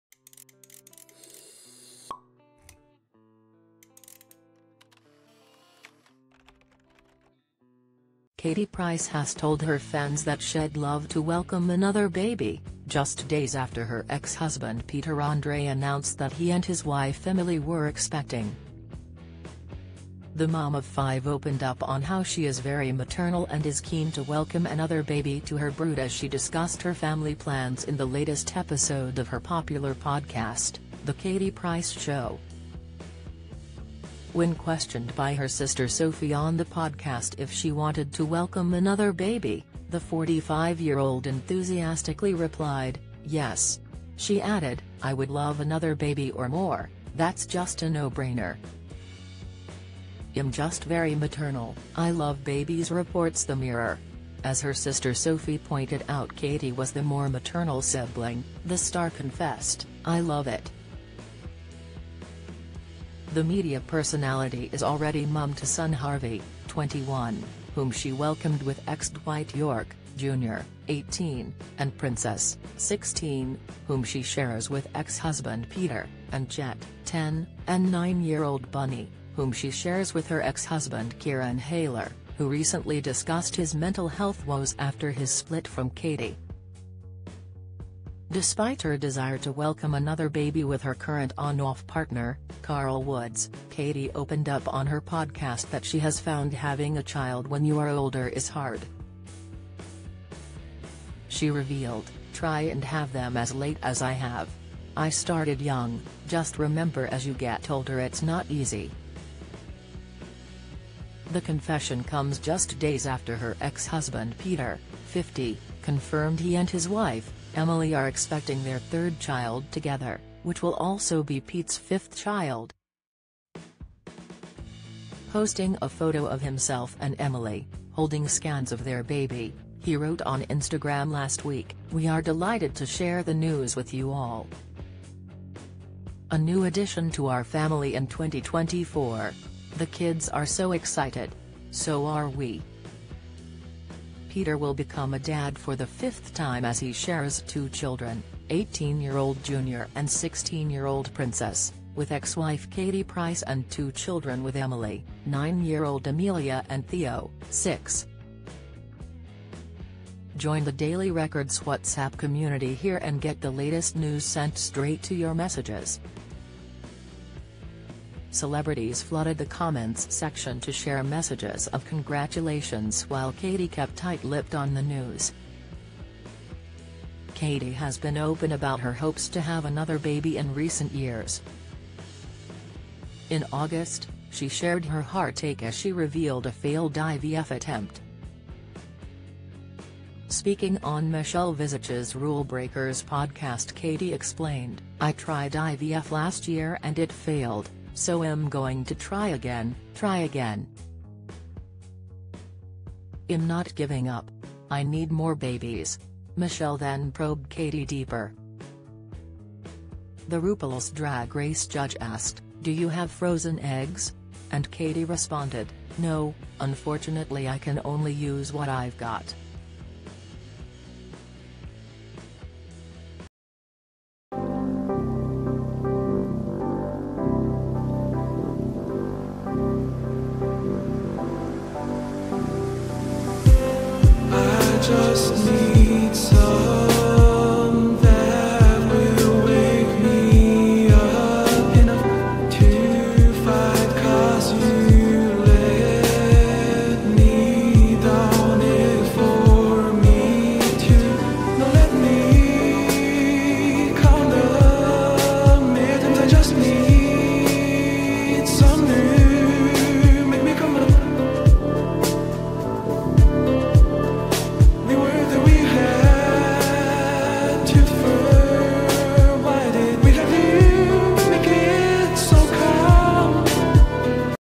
Katie Price shares baby update following Peter and Emily's pregnancy reveal11583023
Katie Price shares baby update following Peter and Emily's pregnancy reveal